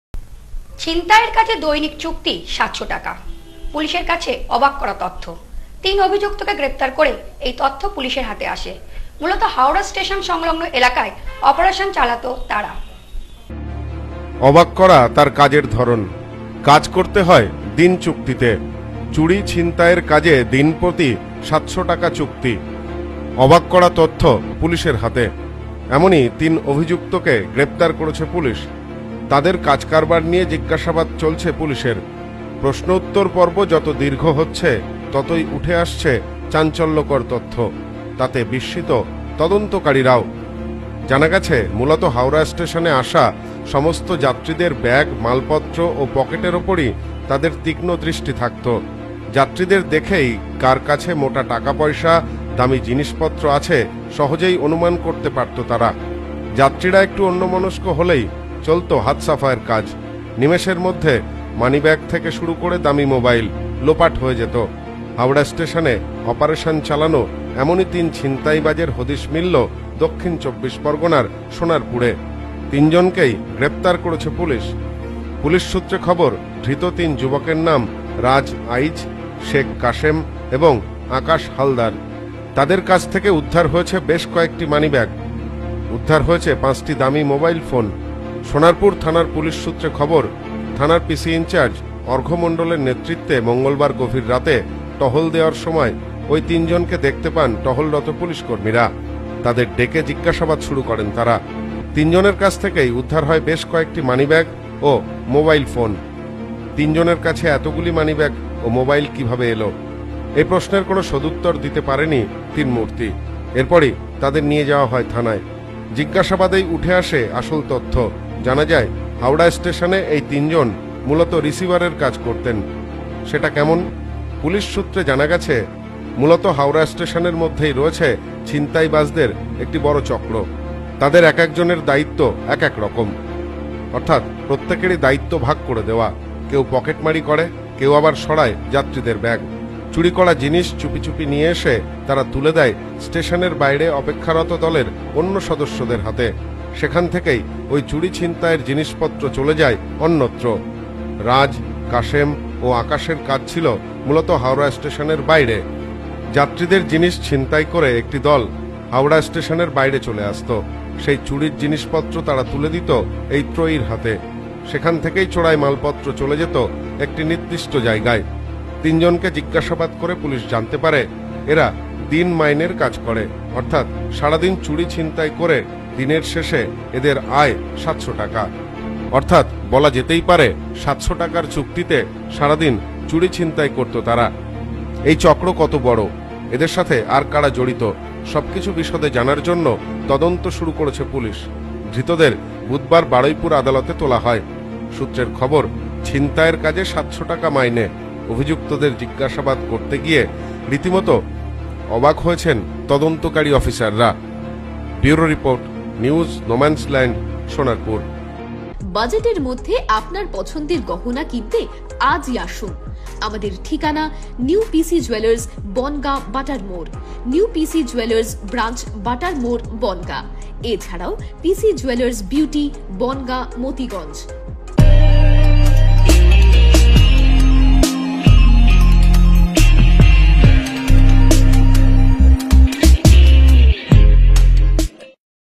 "ছিনতাইয়ের" কাজ দৈনিক চুক্তি ৭০০ টাকা?
"ছিনতাইয়ের" কাজ দৈনিক চুক্তি ৭০০ টাকা। পুলিশি জেরায় অবাক করা তথ্য। তিন অভিযুক্ত কে গ্রেফতার করে এই তথ্য হাতে এসেছে পুলিশের। অভিযুক্তরা মূলত হাওড়া স্টেশনে "অপারেশন" চালাত বলে খবর।